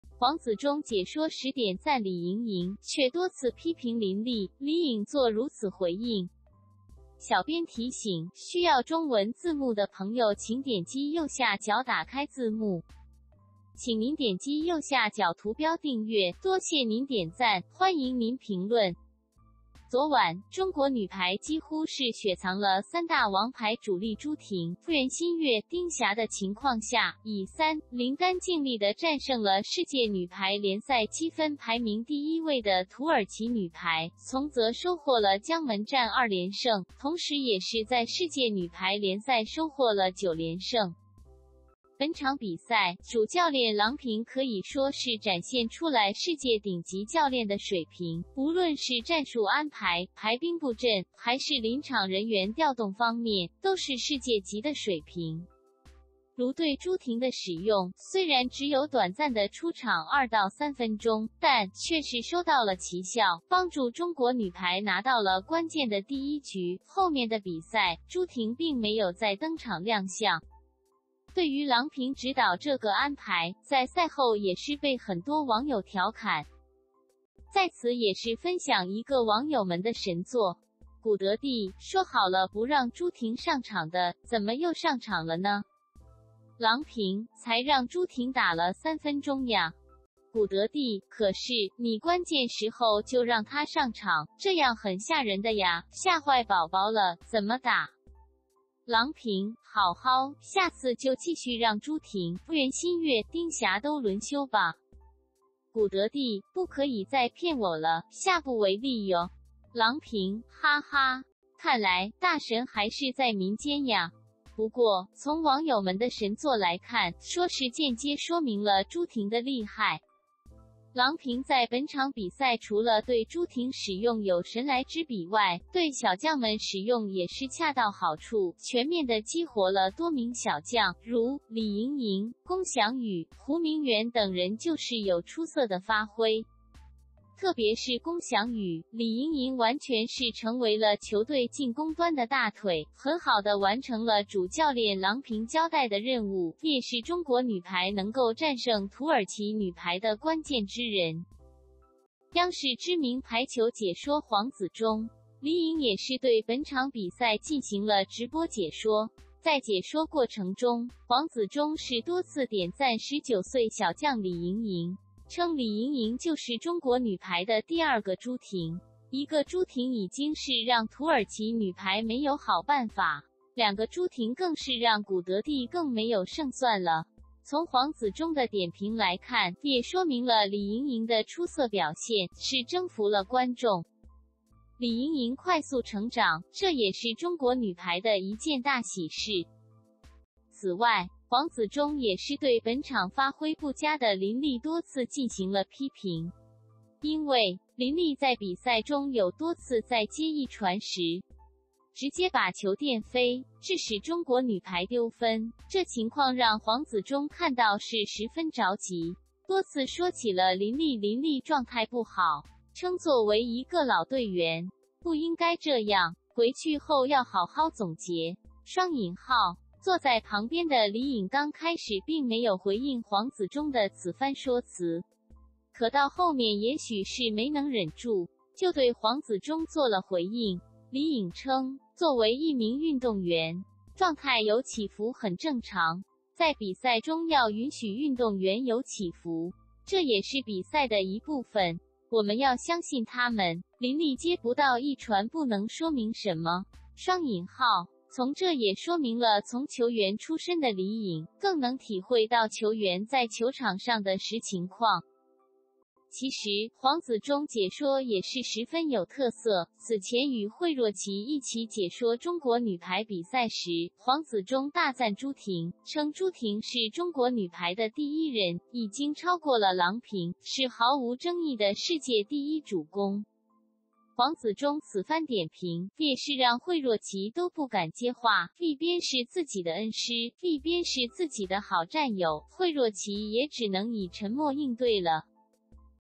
黄子忠解说时点赞李盈莹，却多次批评林莉，李颖作如此回应
昨晚，中国女排几乎是雪藏了三大王牌主力朱婷、袁心玥、丁霞的情况下，以3：0干净利的战胜了世界